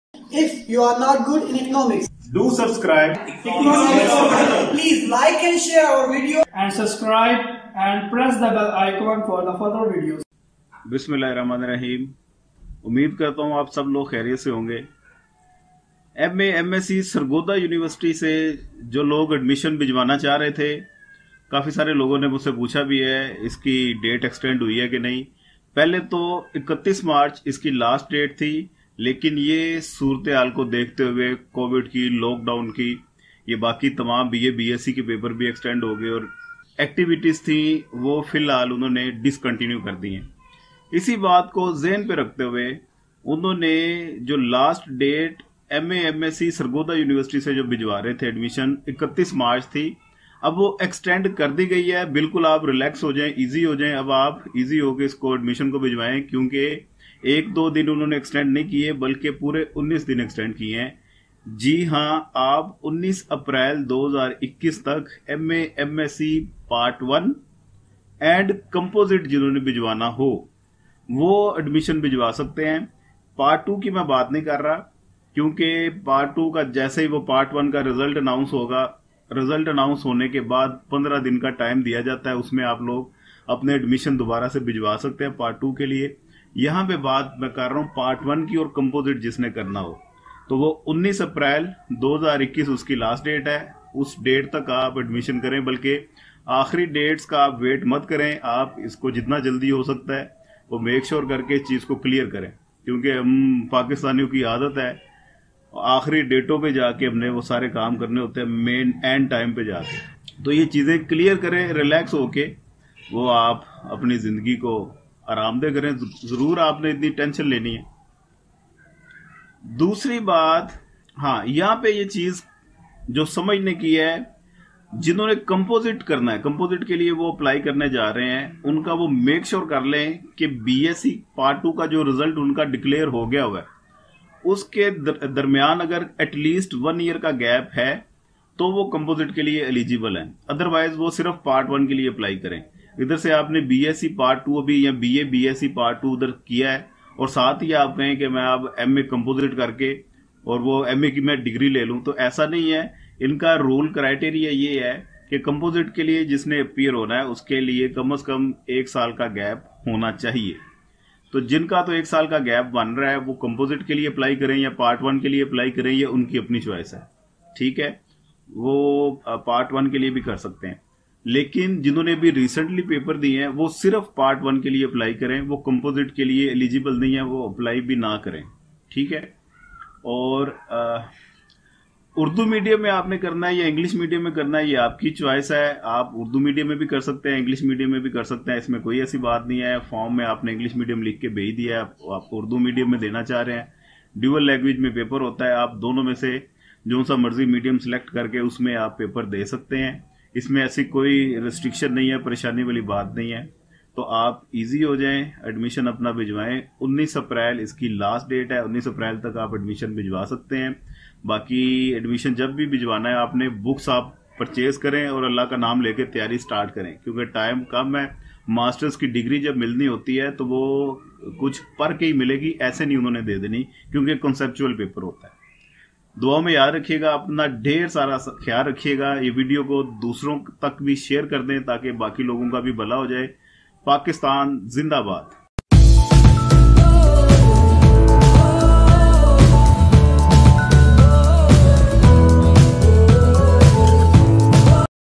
New admission 2021 notification for MA/MSC (Sargodha university)
Salam to all. Great new about the new notification of admission for the students of MA/MSC part 1 or composite is available in this video. Students are worried about the admission date but now they relax. Watch the video to know what is the next date to submit their admission form.
Don't forget to give us your precious feedback. Jazakallah.
Best Regards: Prof. Raees Ahmad (M.Phil Economics)